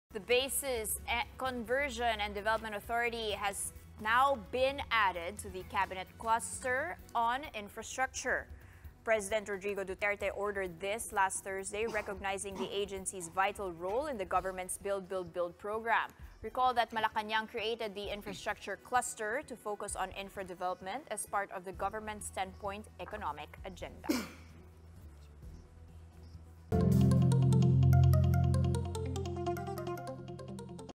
Palace orders BCDA inclusion in Cabinet infra cluster
Recognizing the Bases Conversion and Development Authority's vital role in the government's Build, Build, Build push, Pres. Duterte has ordered the agency's inclusion in the Cabinet cluster on infrastructure.